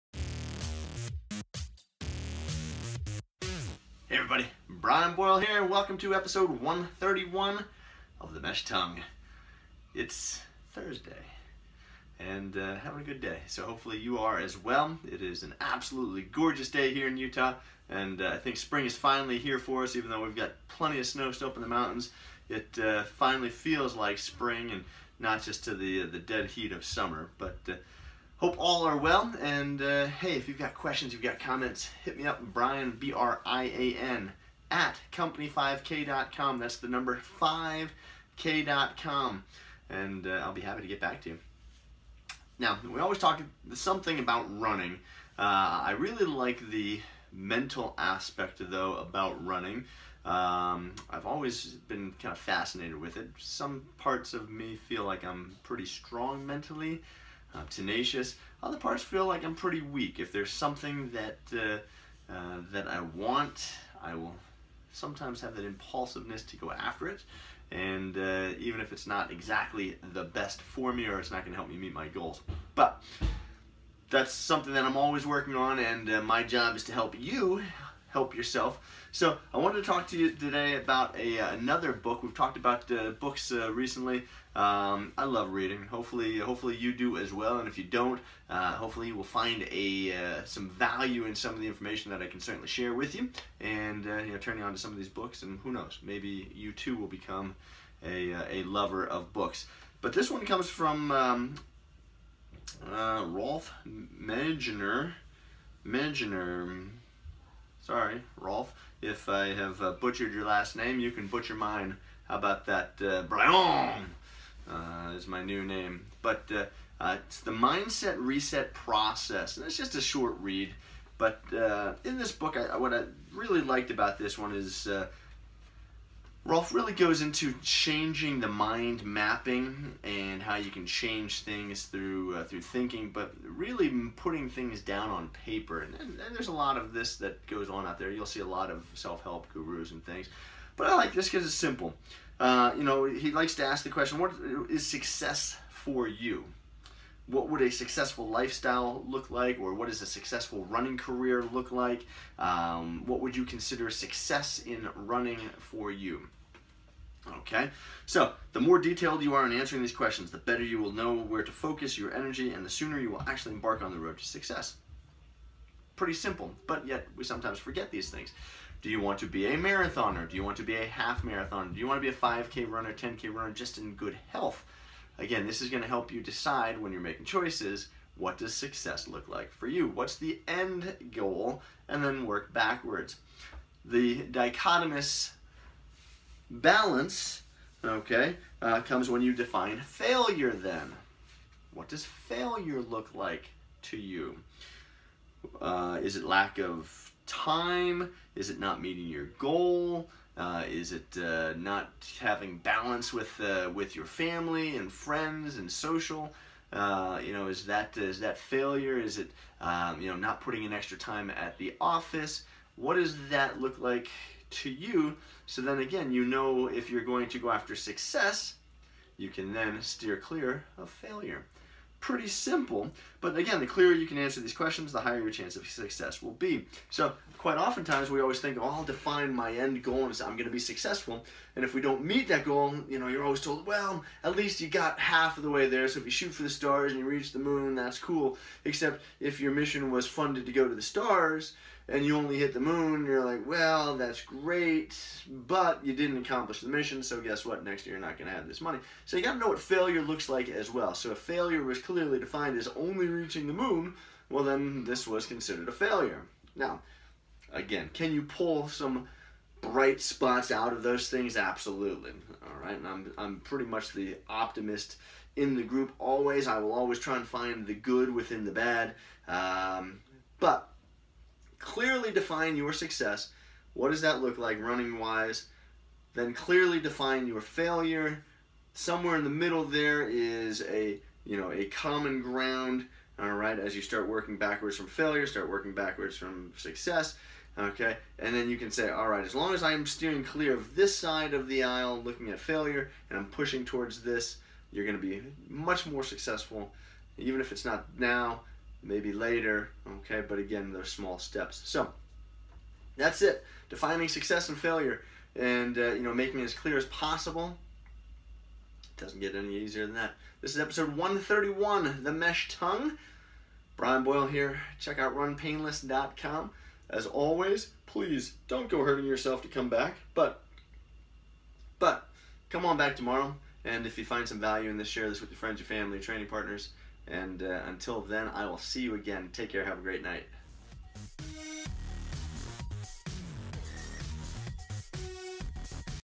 Defining Success And Failure In Running: The Mesh Tongue Episode 131
In Episode 131 of the Mesh Tongue, I discuss the book Mindset Recess Process by Rolf Magener. He talks about clearly defining success and also failure so as you move forward you know exactly what to steer away from and to focus on. For example, want to run a marathon and be successful? Define what not running the marathon looks like to you and how you would feel. Steer clear of any situations or decisions which may lead you to that failure scenario. How do you define success and failure? Leave those comments below.